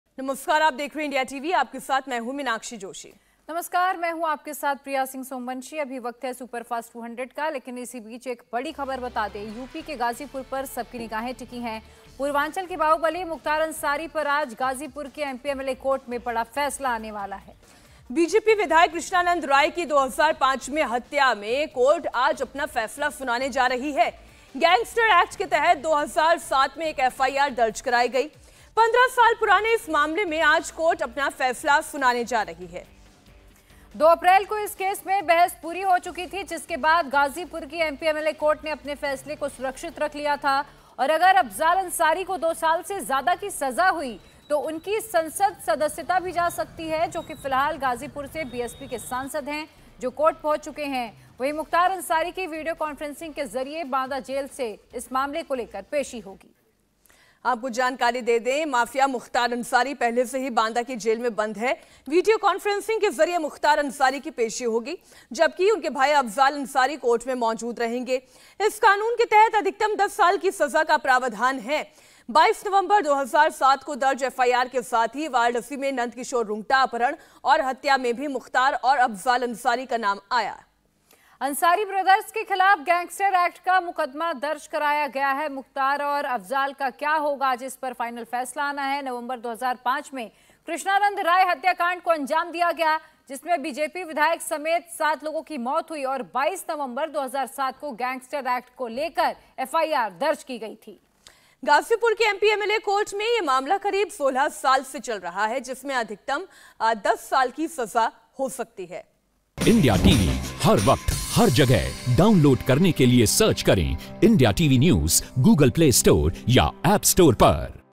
Mukhtar Ansari- Krishnanand Rai News: MP-MLA कोर्ट आज गैंगस्टर एक्ट में फैसला सुनाएगा | Hindi News
BJP विधायक Krishnanand Rai  की 2005 में हत्या में कोर्ट अपना फैसला सुनाएगी.. गैंगस्टर एक्ट के तहत 2007 में एक FIR दर्ज कराई गई.. 15 साल पुराने इस मामले आज कोर्ट अपना फैसला सुनाने जा रही है.. 2 अप्रैल को इस केस में बहस पूरी हो गई थी.. जिसके बाद गाजीपुर की MP-MLA कोर्ट ने फैसला सुरक्षित रख लिया था.. अगर अफजाल अंसारी को 2 साल से ज्यादा की सजा हुई तो उनकी संसद सदस्यता जा सकती है.. जो कि फिलहाल गाजीपुर से बीएसपी के सांसद हैं.. जो कोर्ट पहुंच चुके हैं.. वहीं मुख्तार अंसारी की वीडियो कॉन्फ्रेंसिंग के जरिए बांदा जेल से पेशी होगी..